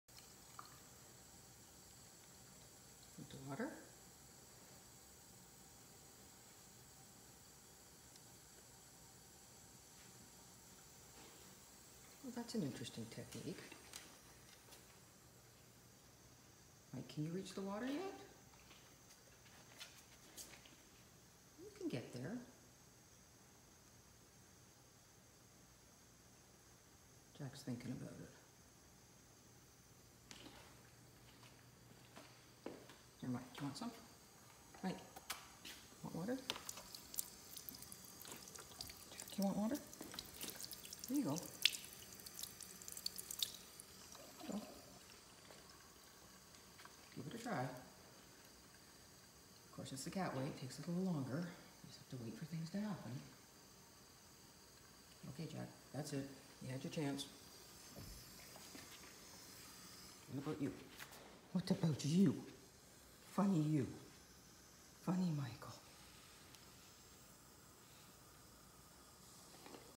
Funny Great Dane Interrupts Cat's Drink From The Sink
Mikey the Great Dane likes to be a part of what his buddy Jack the Cat is doing. Watch and laugh as Mikey comes over to check out Jack enjoying a little drink of tap water, but keeps a respectful distant from Jack's paws.